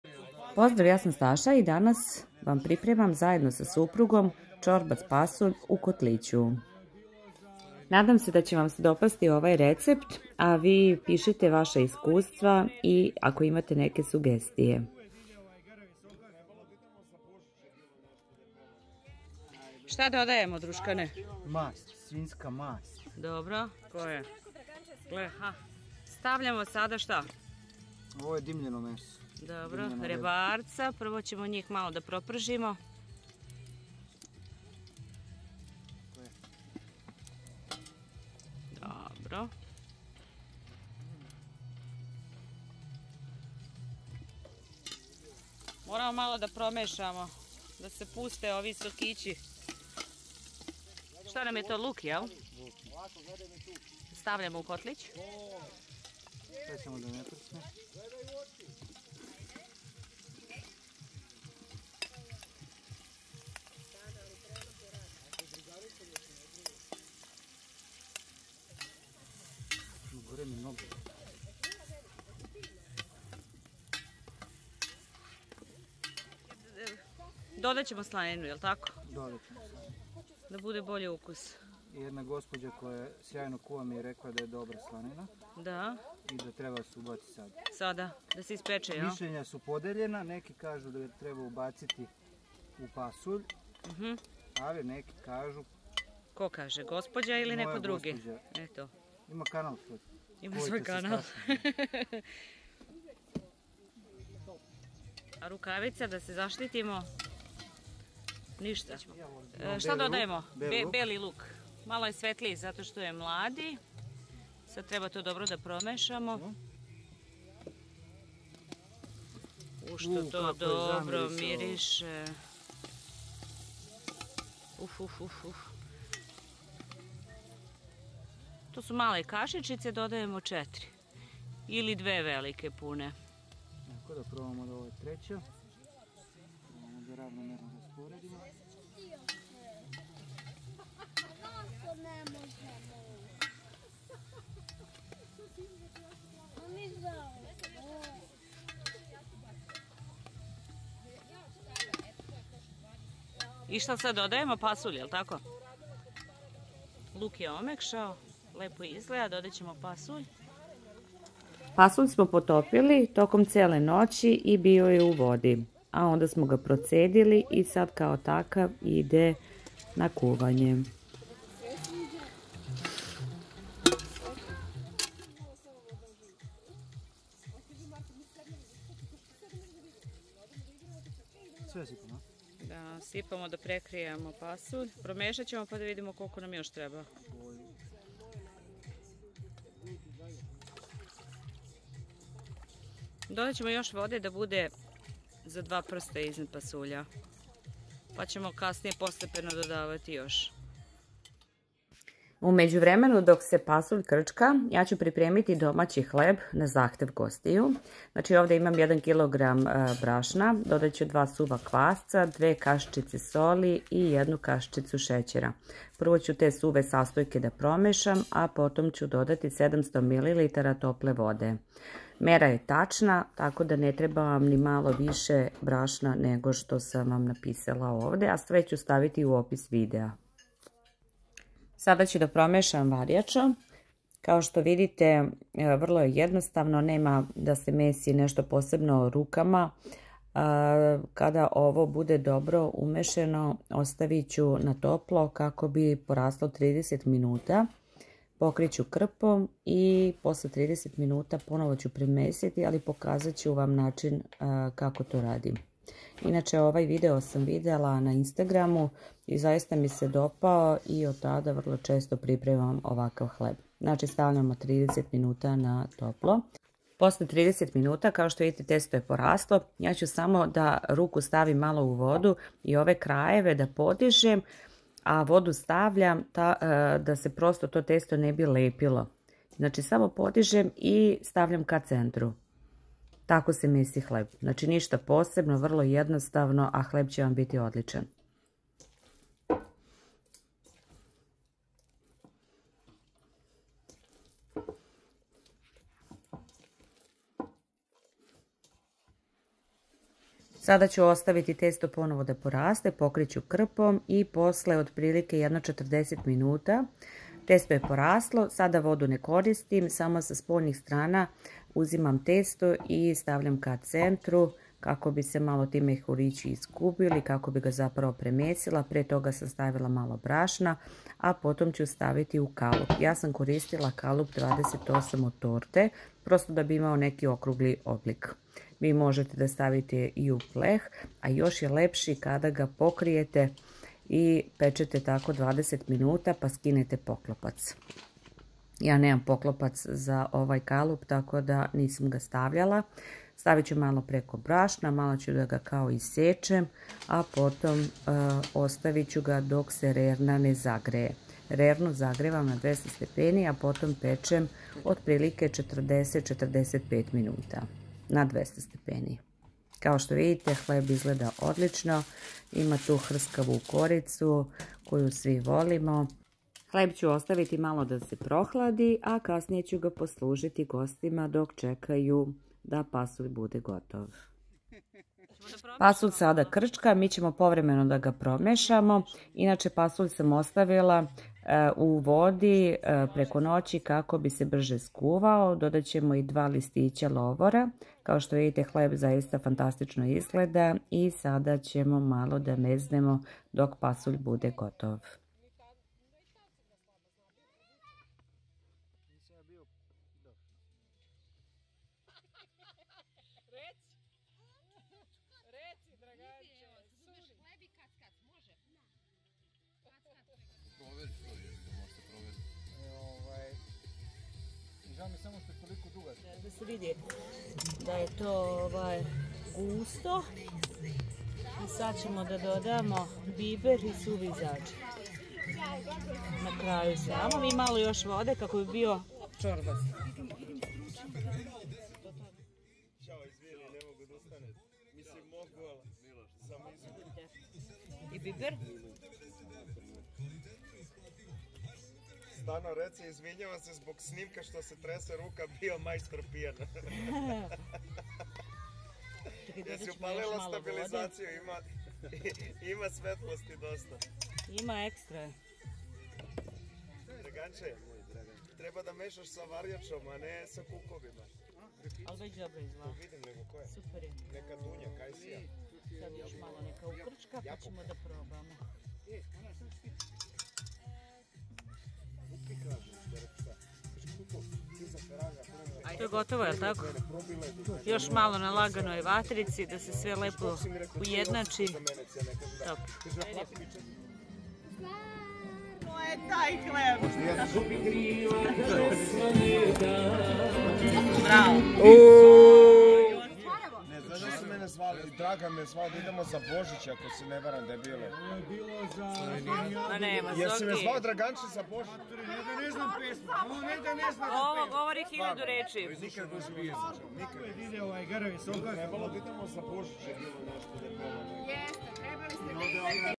Čorbast pasulj u KOTLIĆU, spremamo SUPRUG I JA😃
Čorbast pasulj

Sastojci:

1 200g pasulja Tetovca
600 g crnog luka
suvo meso po želji
3-4 čena belog luka
3-4 kašike masti
4 kašičice  slatke paprike
lovor
suvi začin I biber
voda

Hleb

1 kg brašna
2 kašičice soli
1  kašičice šećera
2 suva kvasca
700 ml tople vode